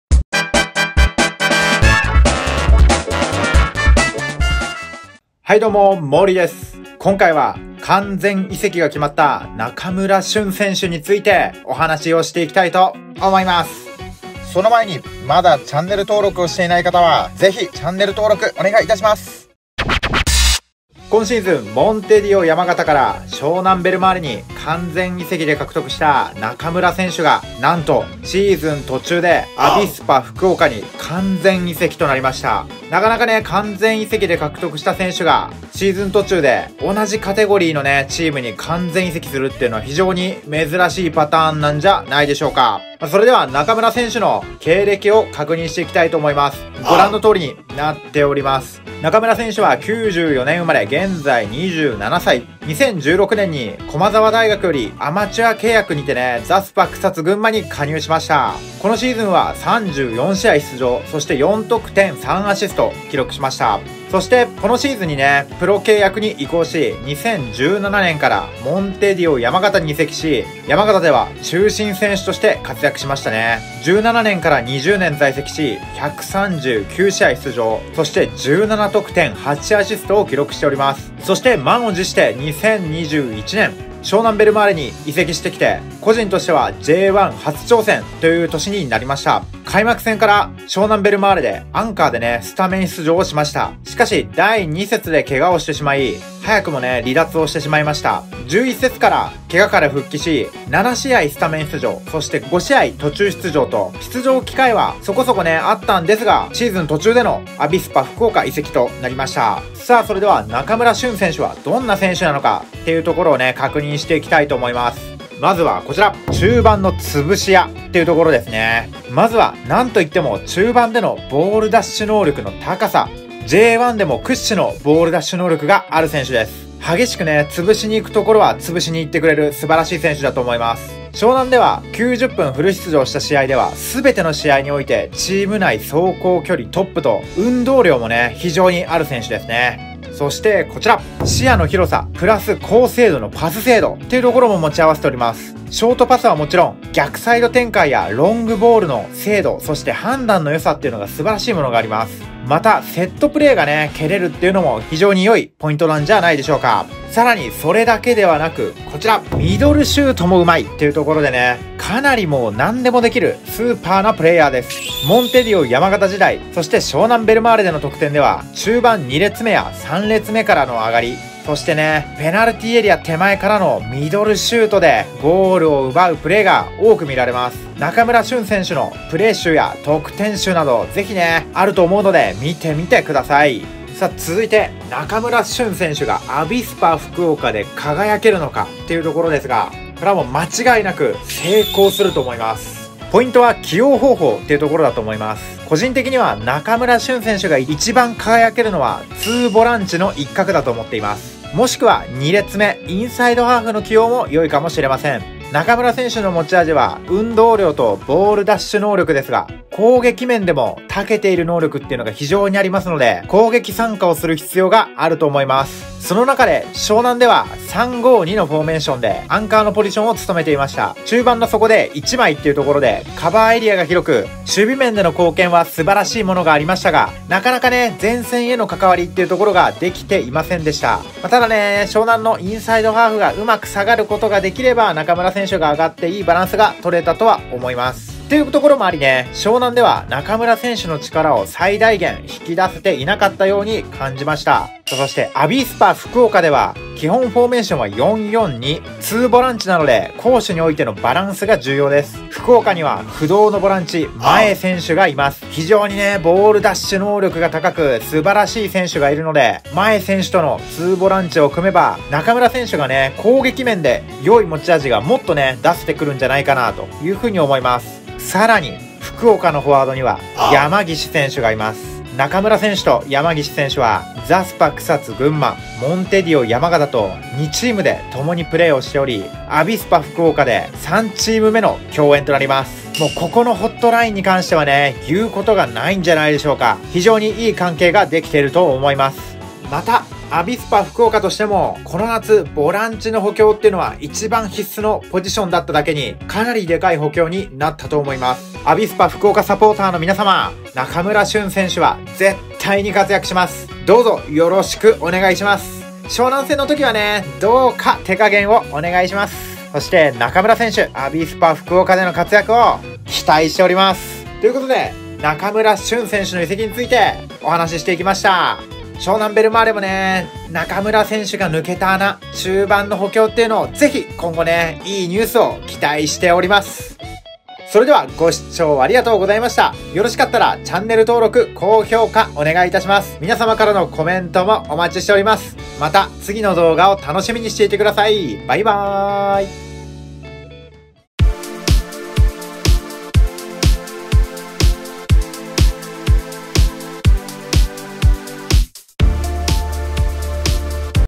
中村駿 アビスパ福岡へ完全移籍！活躍できるのか？（湘南ベルマーレ・選手紹介）
湘南ベルマーレからアビスパ福岡へ完全移籍となった中村駿選手
アビスパ福岡サポーターへ簡単な選手紹介動画となっています！
移籍先での活躍を期待しております！